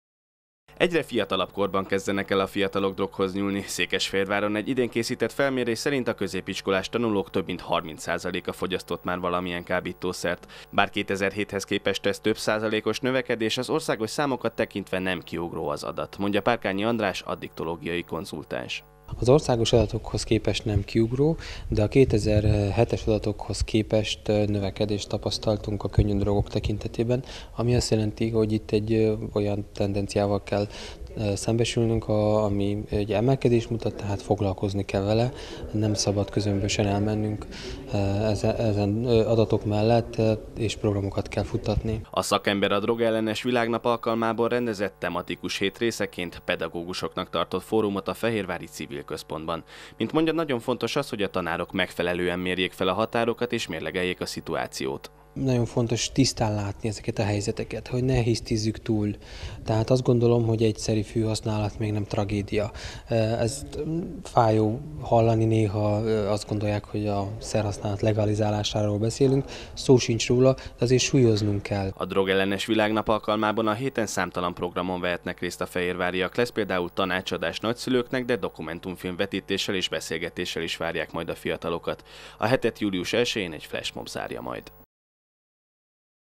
Drogról pedagógusoknak
Június 26-án volt a drog ellenes világnap. Székesfehérváron ez alkalomból tematikus programok várnak minden érdeklődőt ezen a héten. Ma pedagógusoknak beszélt a problémáról egy addiktológus, de lesz tanácsadás, filmvetítés és interaktív beszélgetés is a következő napokban.